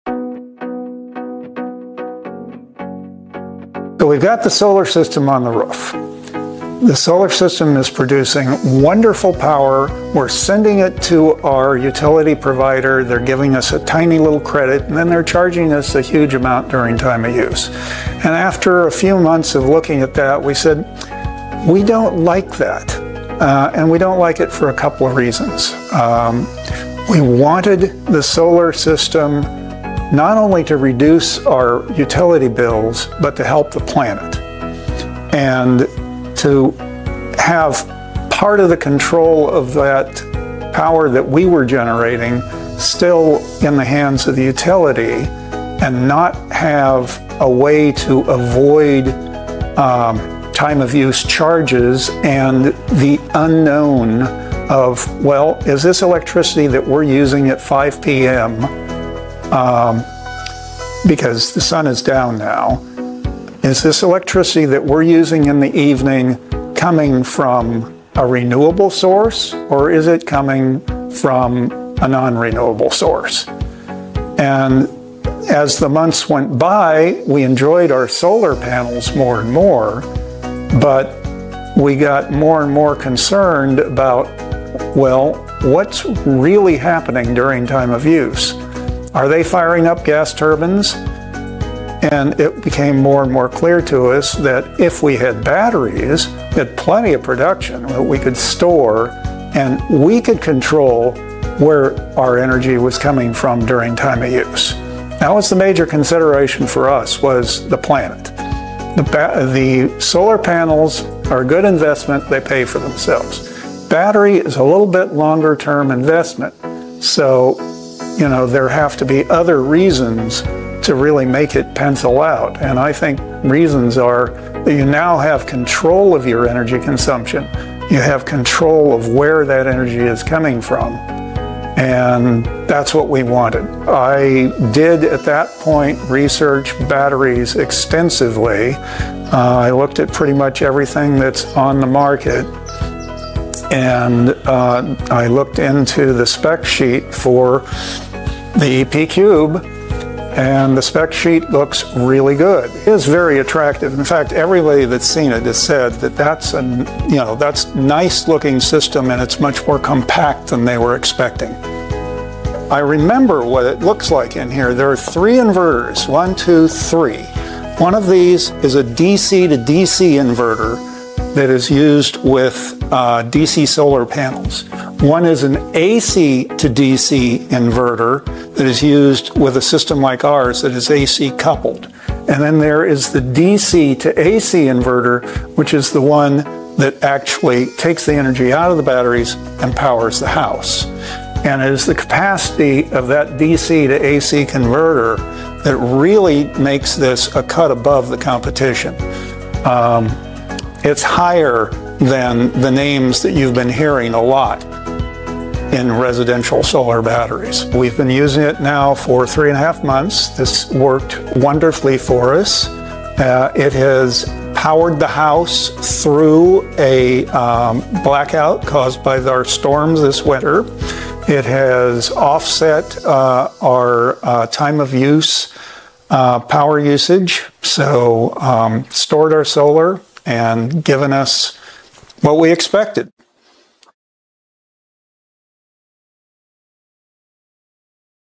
EP Cube Energy Storage System - User: Power the house, Reduce Utility Bills and Help the Planet
Doug has the residential energy storage system EP Cube in his house in California, he said "We wanted the solar system not only to reduce our utility bills but to help the planet." watch this video to get how the EP Cube benefited his family.